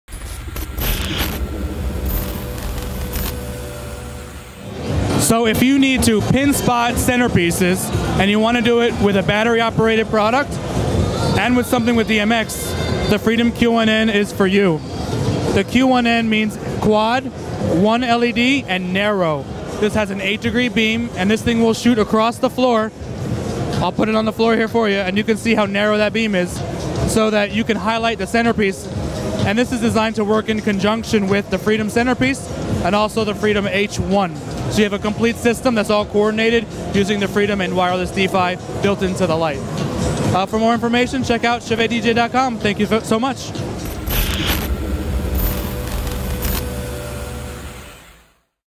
ChauvetDJ Freedom Q1N RGB+WW Pin Spot with D-Fi Wireless Connection | Disc Jockey News
Freedom Q1N is a battery-powered RGB+WW pin spot fixture with a built-in D-fi transceiver. Part of the 100% TRUE wireless Freedom ecosystem, Freedom Q1N can be controlled remotely and programmed in sync with other Freedom products. This compact and powerful fixture features a narrow beam perfect for highlighting special event elements. Its magnetic base and small size mean effortless mounting, controlling, and transporting. The Freedom Q1N system includes 4 fixtures, a carry bag, multi-charger, and IRC-6 remote.